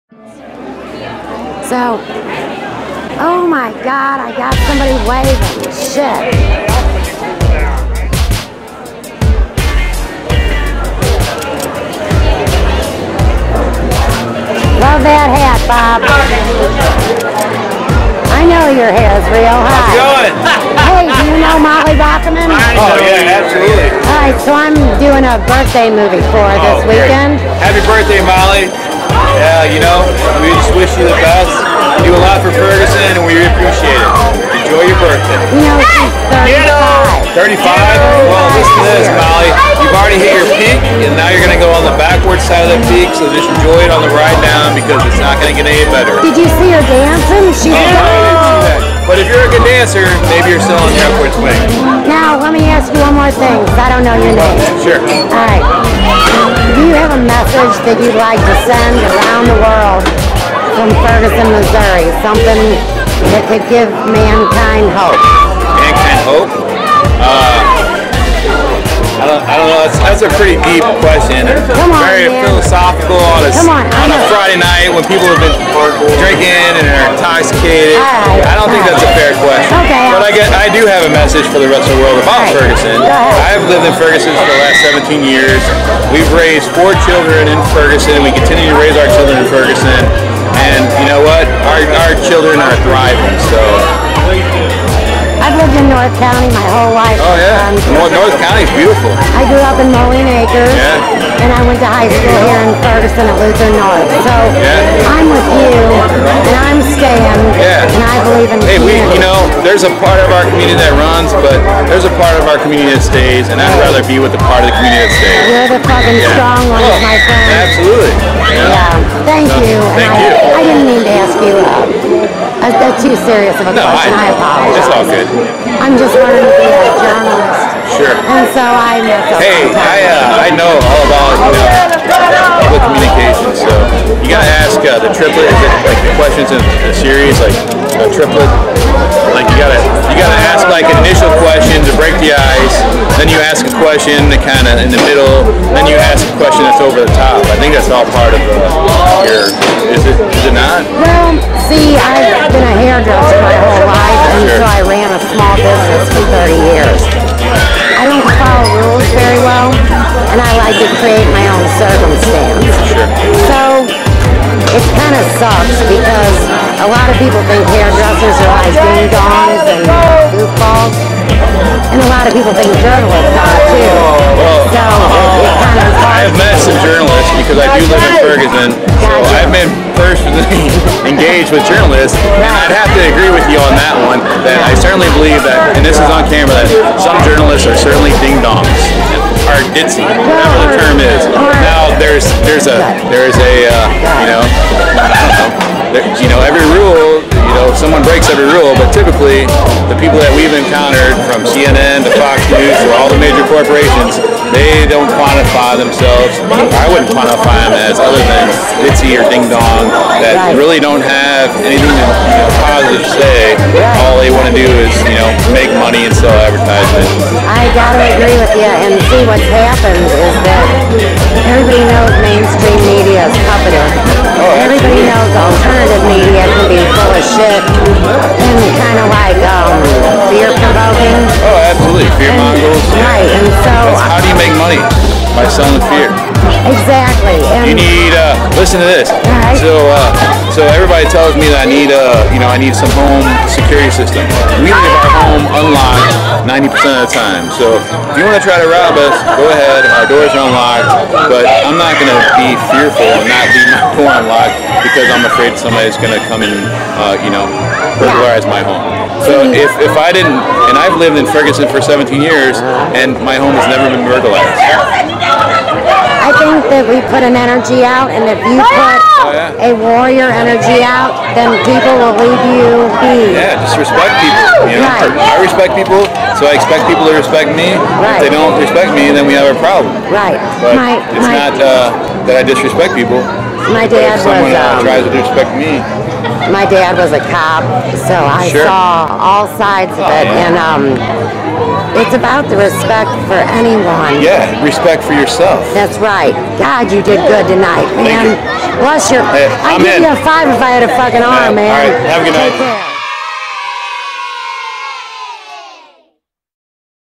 Talk on the Wild Side in Ferguson
Met a new friend at 501 concert venue who wanted to say Happy birthday to Molly from EarthDance Organic Farm and speak up a little bit about how unfair mainstream media is in today's world.  Ferguson is Fun if you don't freak out.